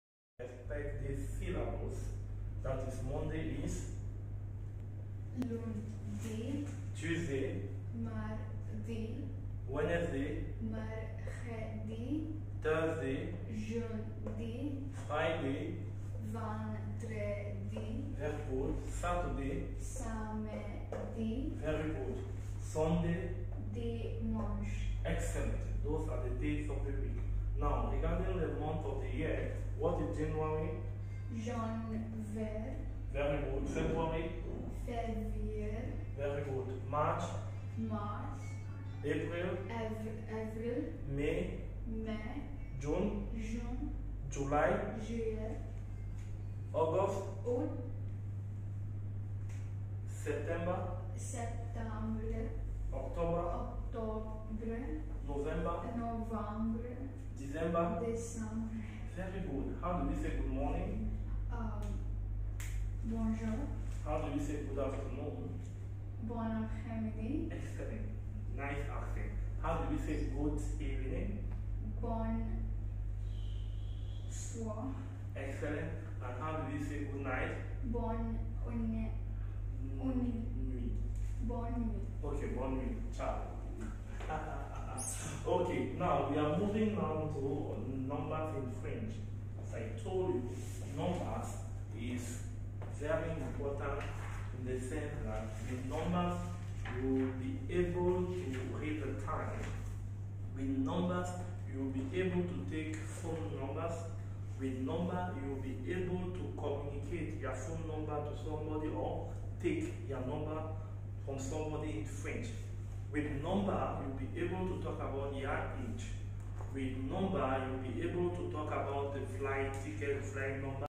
Best French lessons @Spoton Training Institute Dubai
We develop your speaking,listening,reading&writing skills.We have  morning,afternoon,evening&weekend classes. Spoton Provides comfortable&dynamic environment. Enrolment started for an upcoming batch.